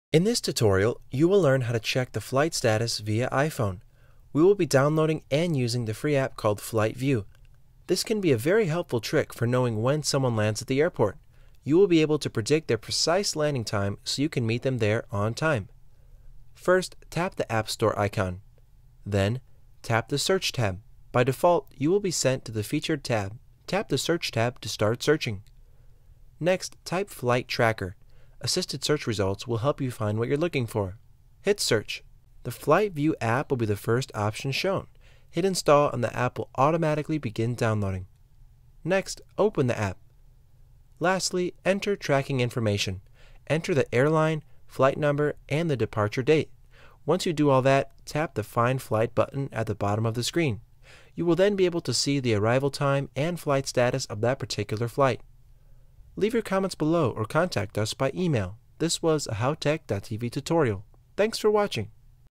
How to Check Flight Status via iPhone and iPad
In this tutorial you will learn how to check the flight status via iPhone.

This tutorial will show you how to check flight status via iPhone. We will be downloading and using the free app called FlightView. This can be a very helpful trick for knowing when someone lands at the airport. You will be able to predict their precise landing time, so you can meet them there on time.

Step #1 -- Tap App Store Icon

Step #2 -- Tap the "Search" Tab

By default, you will be sent to the featured tab. Tap the search tab to start searching.

Step #3 -- Type "flight tracker"

Assisted search results will help you find what you're looking for. Hit search!

Step #4 -- Tap Install

The FlightView App will be the first option shown. Hit install and the app will automatically begin downloading.

Step #5 -- Open the app

Step #6 -- Enter Tracking Information

Enter the airline, flight #, and the departure date. Once you do all that, tap the "Find Flight" button at the bottom of the screen. You will then be able to see the arrival time and flight status of that particular flight.